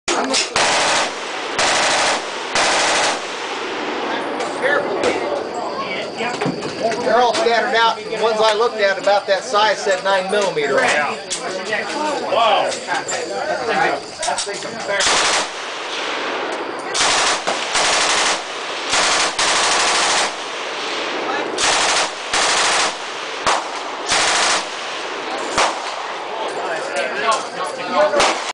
Shooting PKM at Knob Creek
Shooting a PKM on the lower range at the KCR machine gun shoot.

It was my first time shooting something full auto so i didnt really know what to expect.

And i realize all you can really see is the muzzle flash.